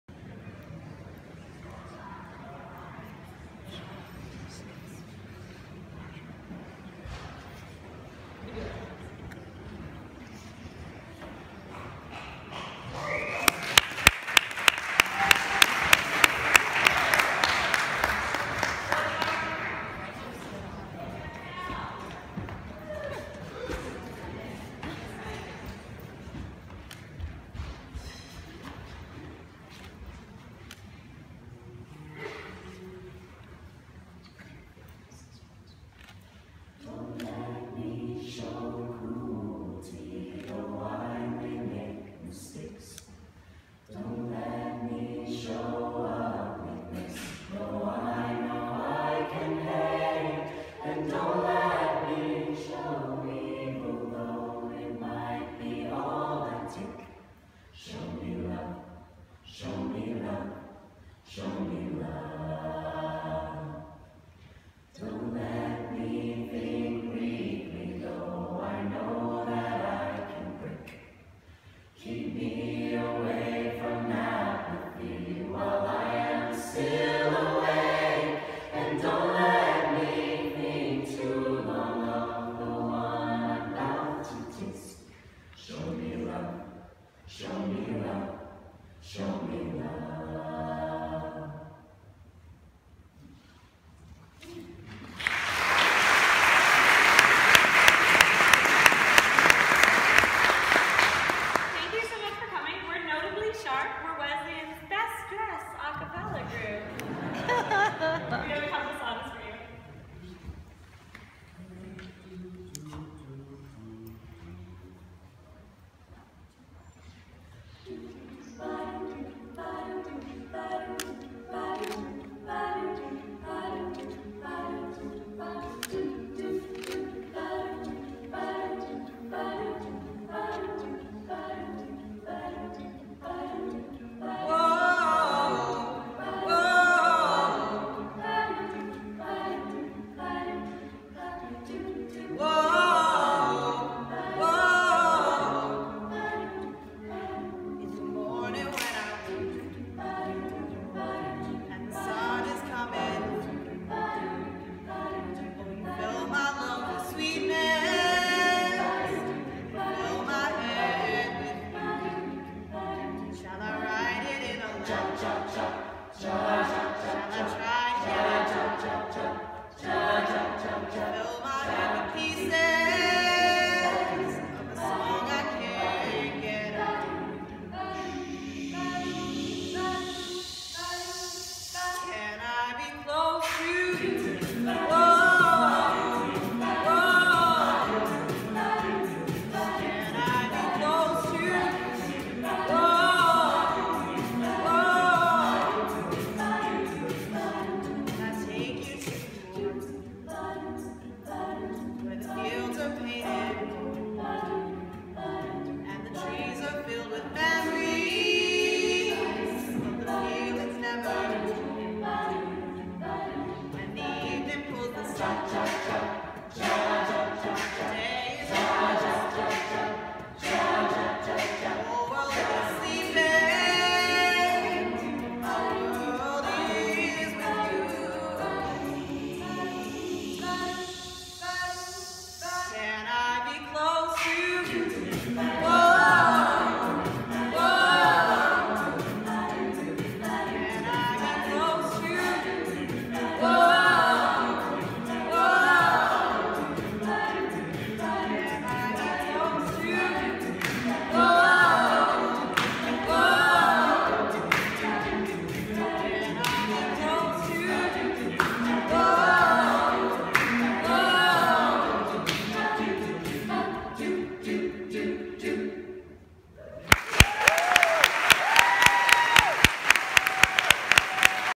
Wesleyan's Notably Sharp in Concert
October 2016 Family Weekend a Capella performance at Memorial Chapel. Middletown, CT